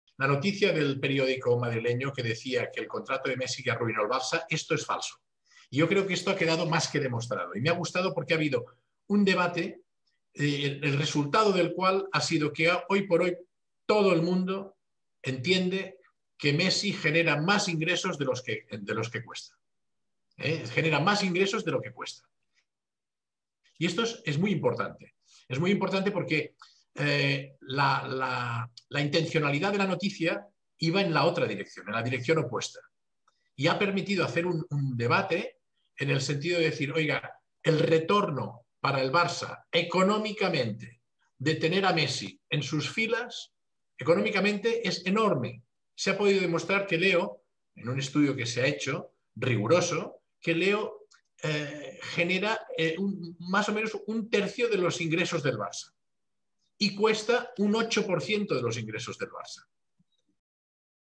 Laporta: "Messi le da ganancias al Barcelona"
Joan Laporta, candidato a presidente del Barcelona, se refirió a las ganancias que genera Lionel Messi en el conjunto catalán.